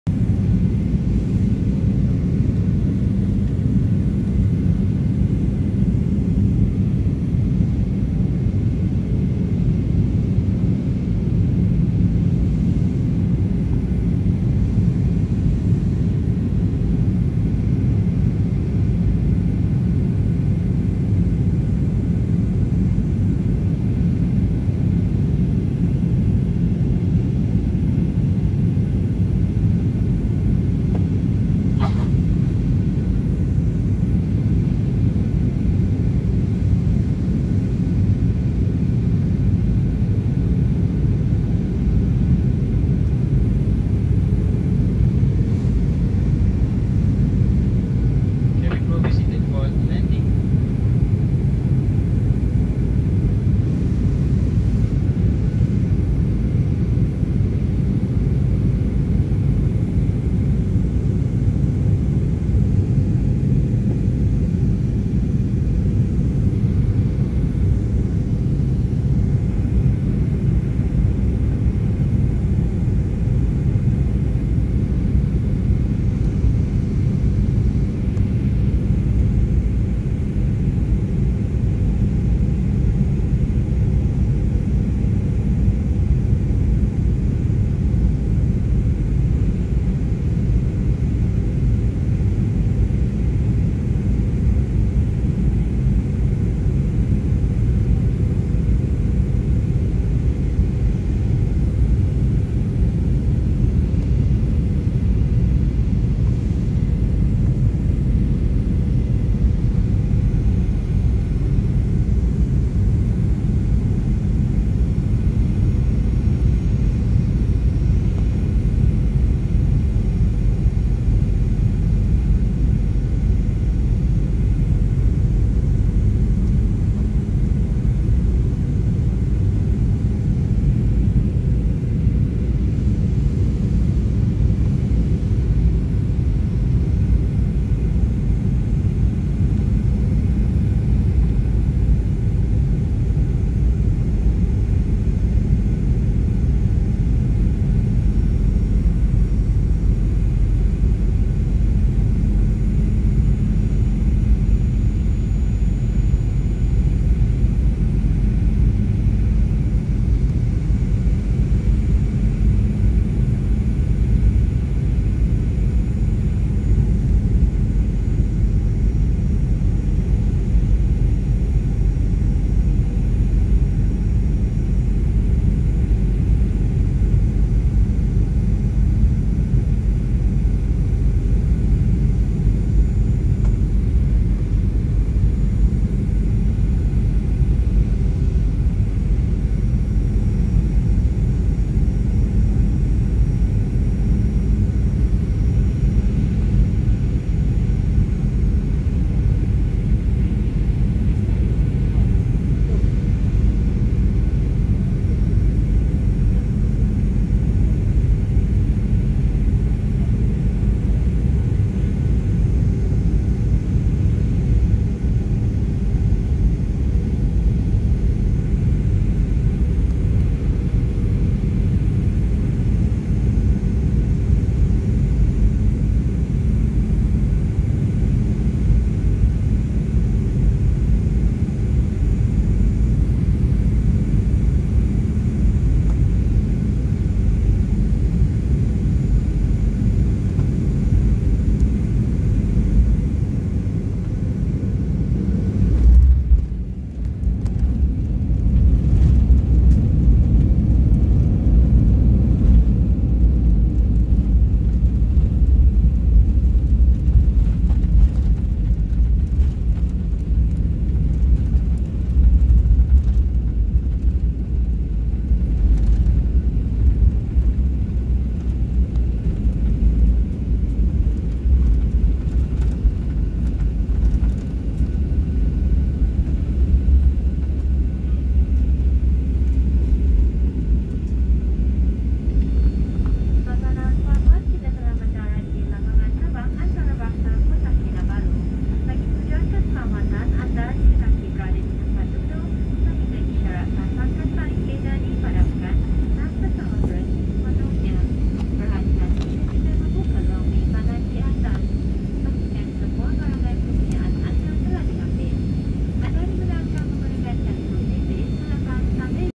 Kota Kinabalu, view from plane until landing
my flight to Kota Kinabalu on 23 Nov 2020, my father passed away on 27 Nov 2020 from complications after a fall. He was 79 years old.